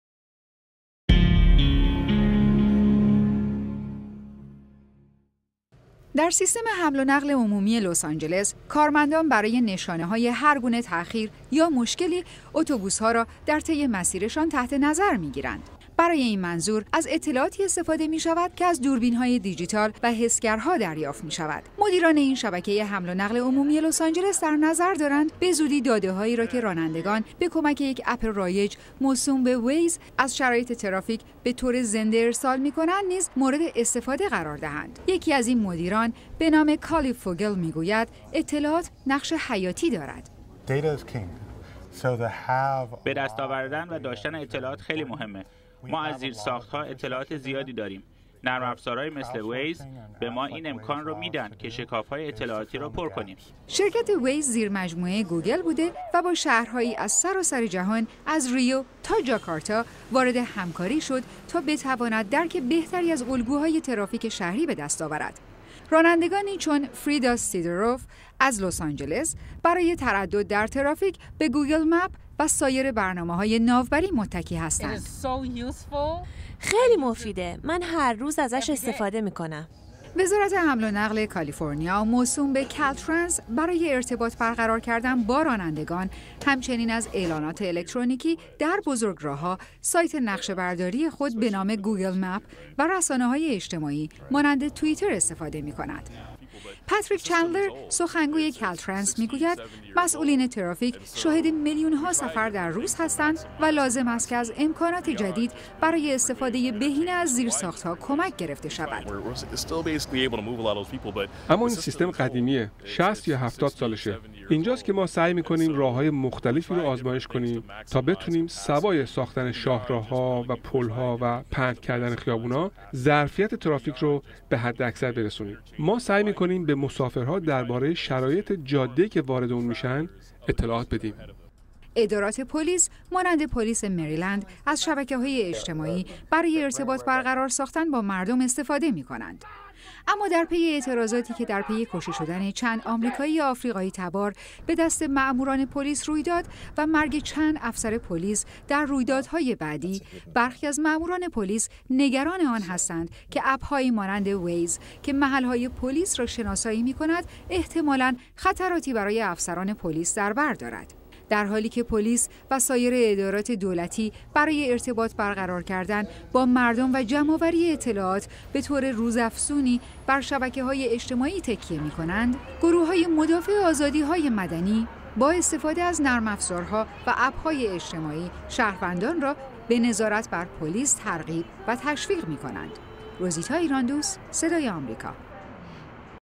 رسانه های اجتماعی ادارات دولتی را دگرگون می‌کنند
رسانه‌های اجتماعی در حال دگرگونی طرز کار ادارات پليس و ساير دواير دولتی‌اند. در حالی‌که رسانه‌های جديد می‌توانند رفتار پليس را زير نظر بگيرند يا در شاهراه‌های شلوغ از حرکت ترافيک اطمينان حاصل کنند، استفاده از آن‌ها بطور روزافزونی زير سوال می‌رود.